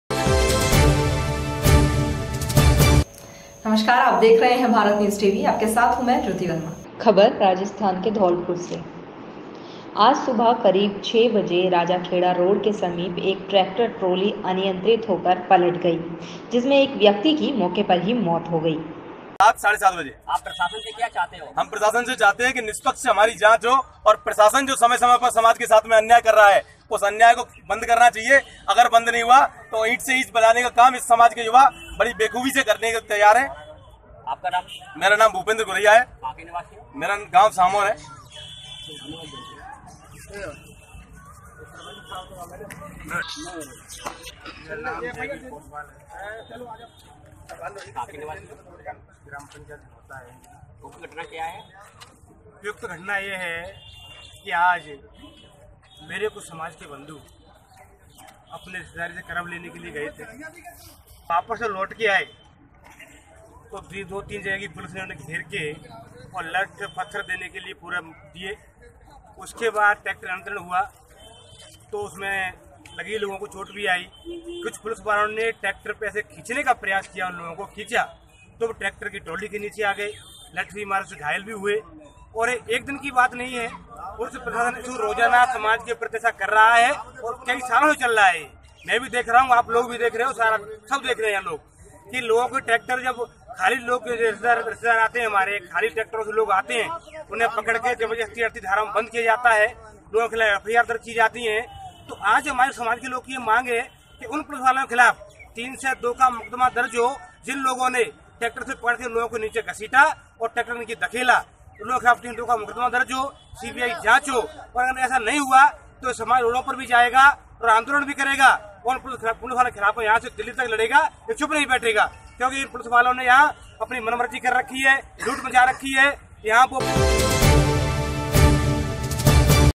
खबर राजस्थान के धौलपुर से | ट्रैक्टर ट्राली पलटने से एक व्यक्ति की हो गई मौत
आज सुबह करीब 6:00 बजे राजाखेड़ा रोड के समीप एक ट्रैक्टर ट्राली अनियंत्रित होकर पलट गया जिसमें एक व्यक्ति की मौके पर ही मौत हो गई ग्रामीणों का कहना है की पुलिस ट्रैक्टर को ना रोको बात ही तो नहीं पलटता और ना ही यह घटना होती सामान्य चिकित्सालय पर मोर्चरी में रखे सबके पास ग्रामीण एकत्र होकर अपनी मांग को लेकर बैठे हुए हैं वही पूरा पुलिस प्रशासन सैकड़ों की तादाद में मौजूद है